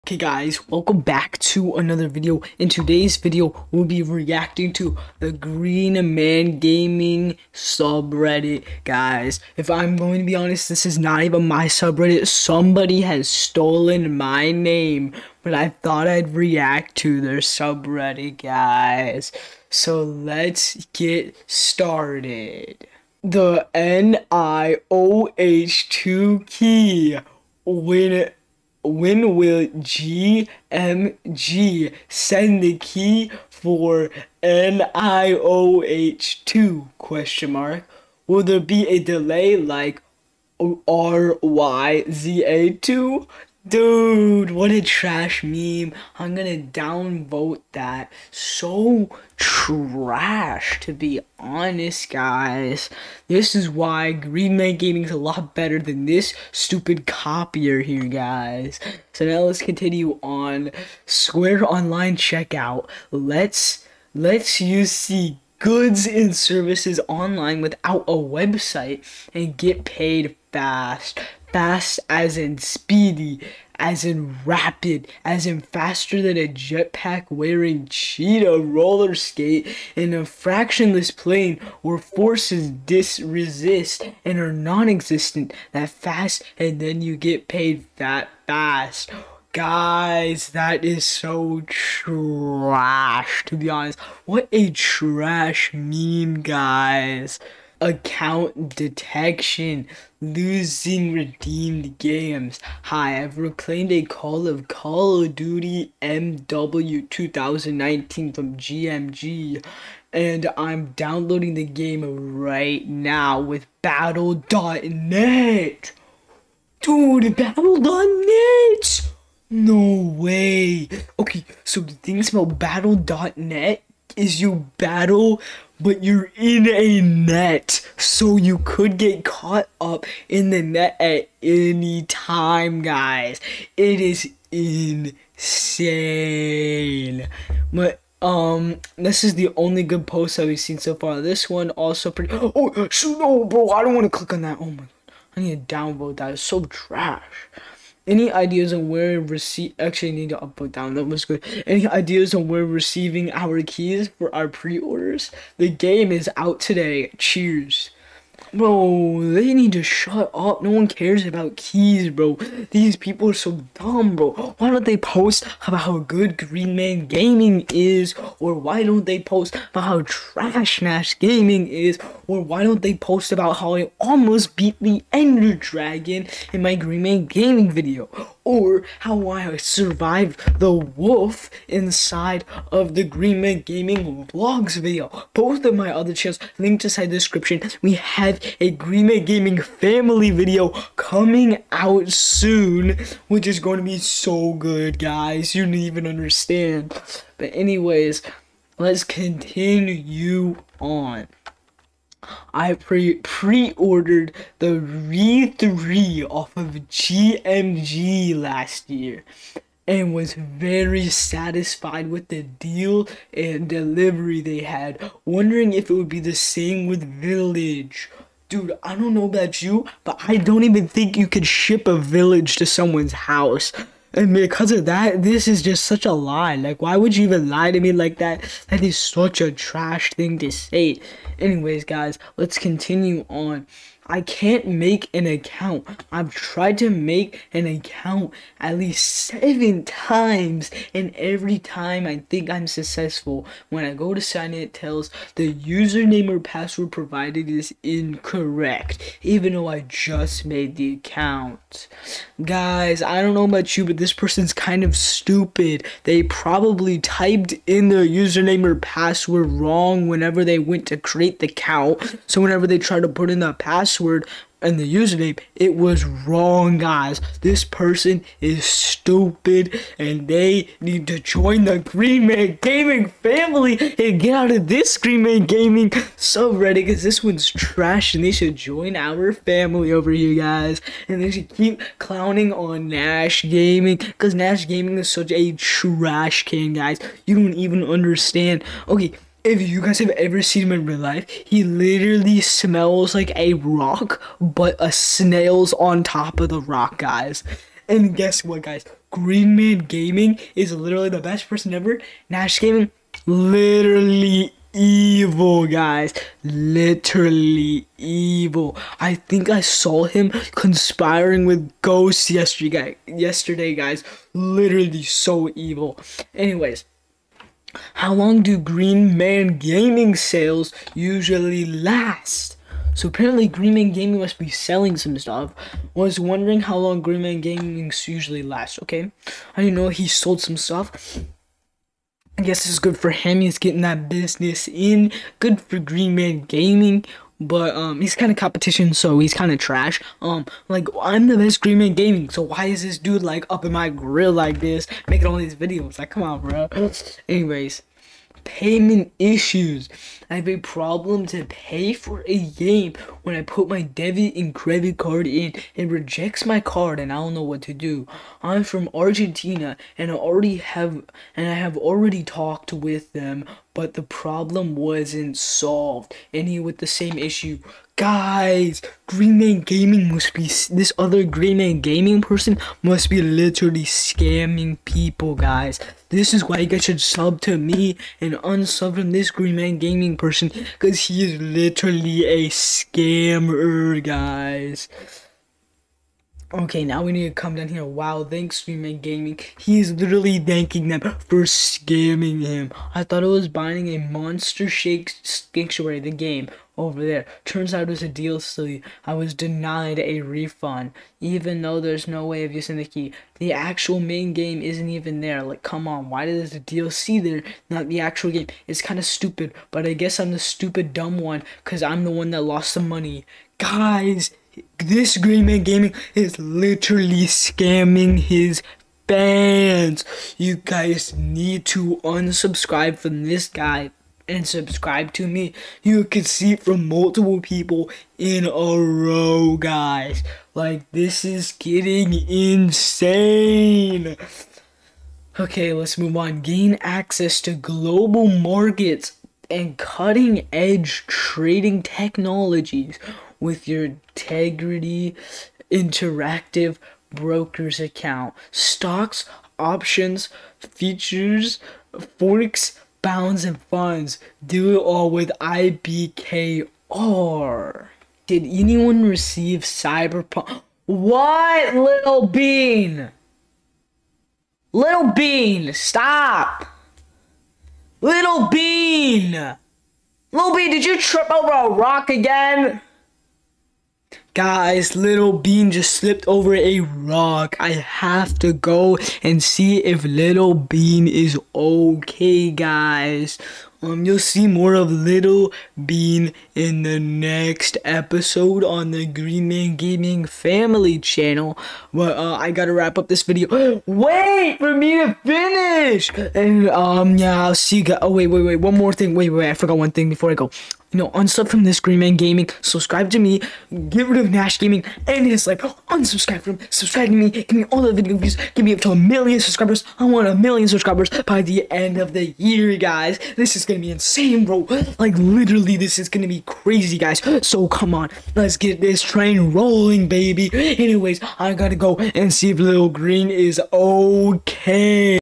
GREEN MAN GAMING REDDIT REVIEW!!!! (SCAMS HIS FANS!?)
Today we reviewed a DIFFERNET GreenManGaming Channel's reddit!
We found out he MIGHT BE SCAMMING IS FANS!!!